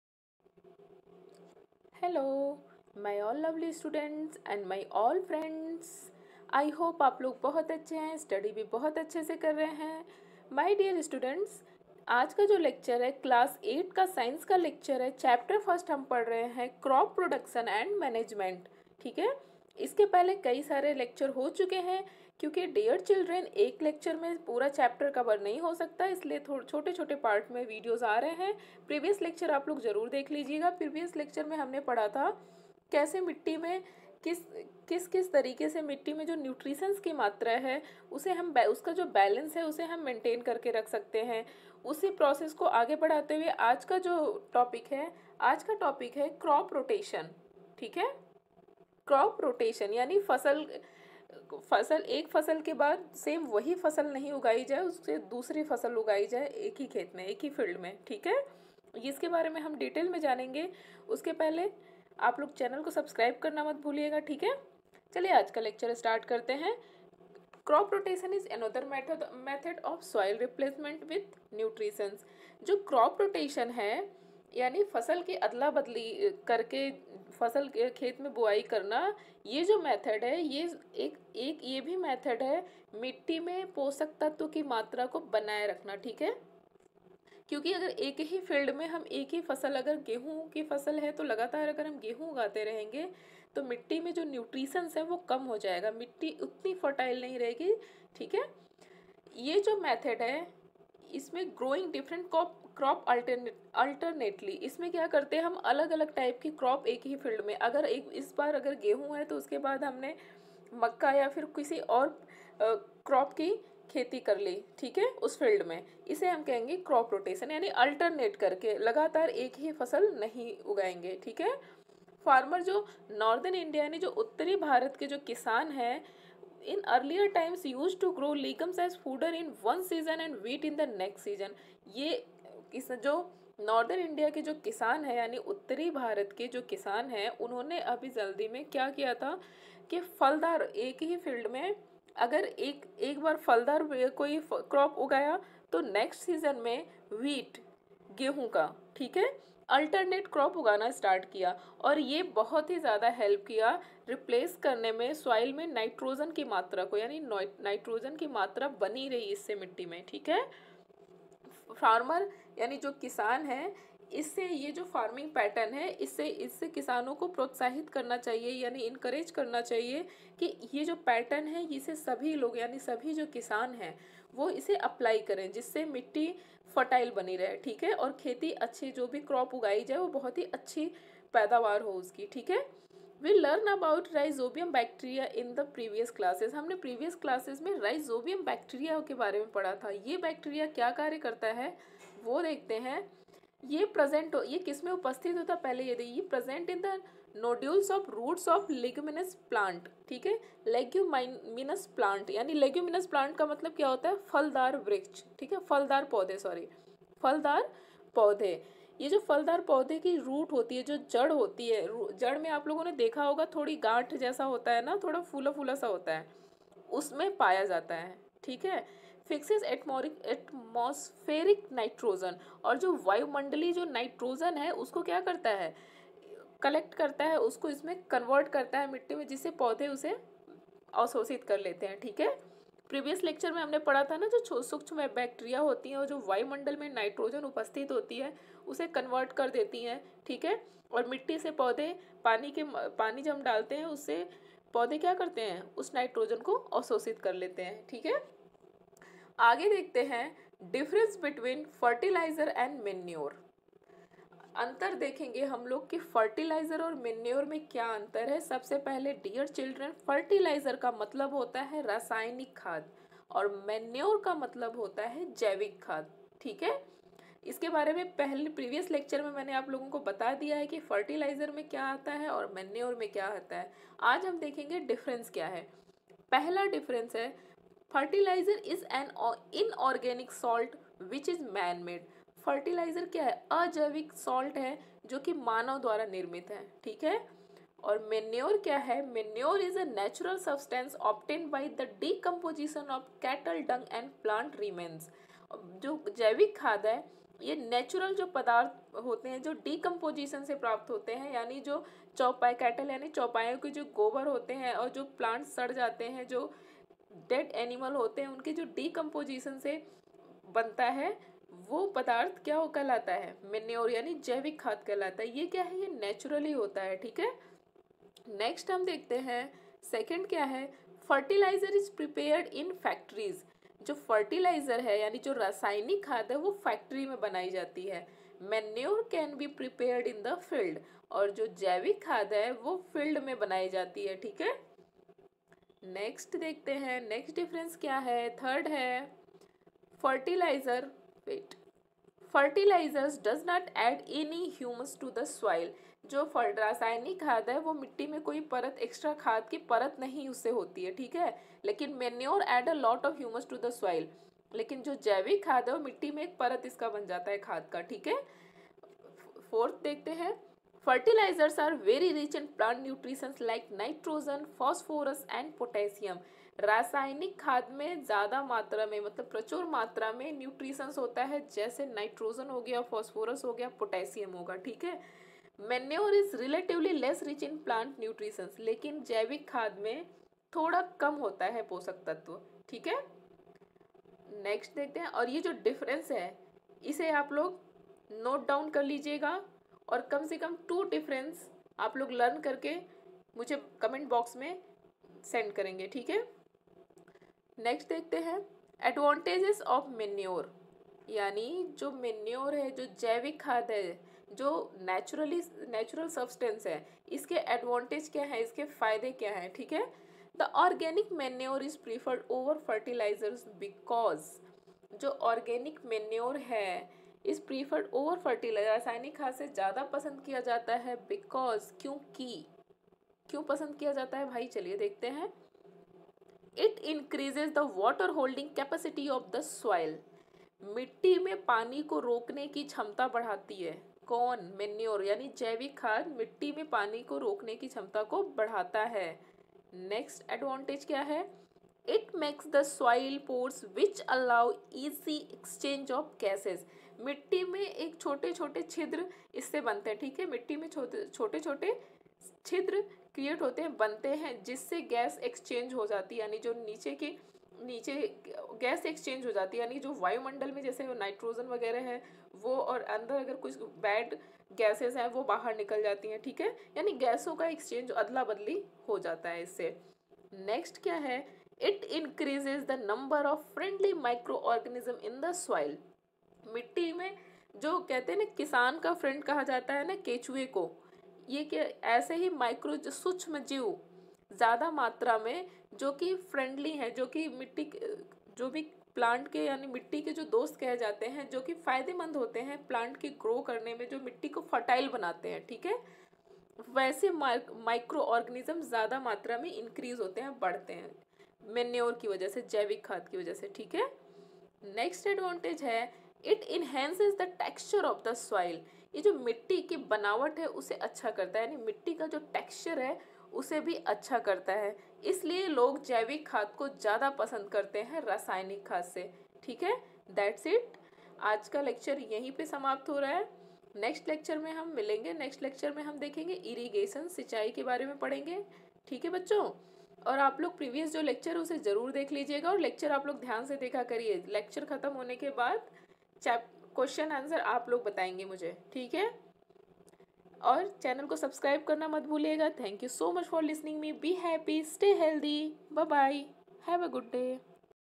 Crop Rotation | Differences between Fertilizer and Manure|खाद और उर्वरक में क्या विशेष अंतर होता है,
Points covered in this lecture :-
1- Crop Rotation
2- Differences between Fertilizer and Manure.
3- Advantages of Manures.
Class-8, Science lecture, chapter-1 .